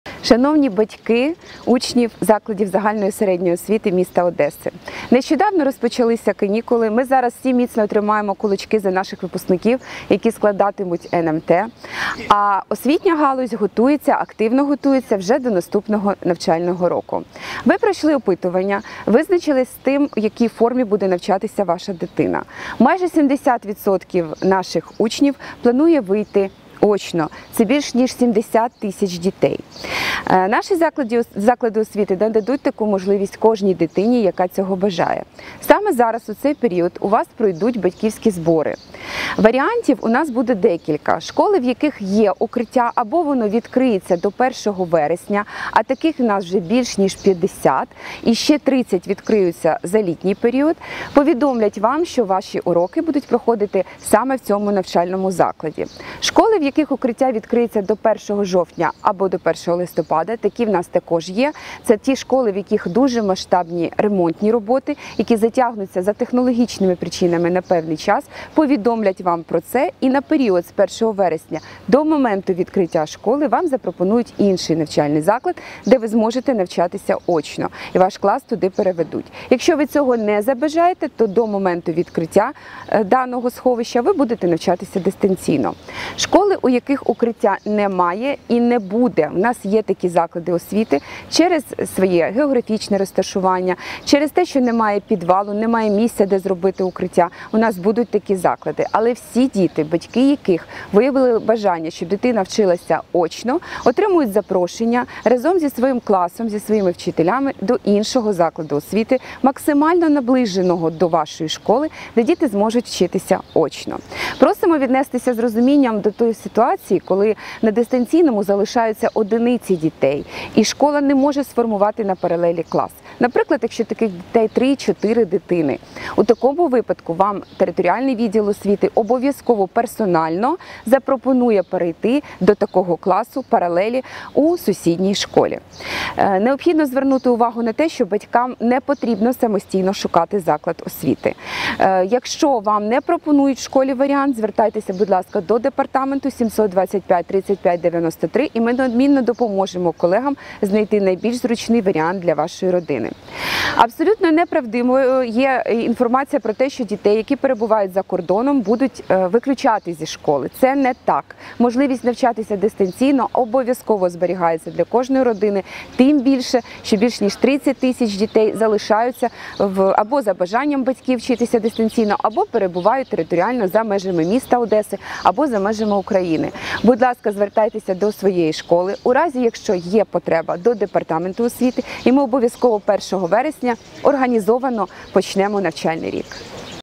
До уваги батьків! Про підготовку до наступного навчального року в Одесі
Коментар директора Департаменту освіти та науки Одеської міськради Олени Буйневіч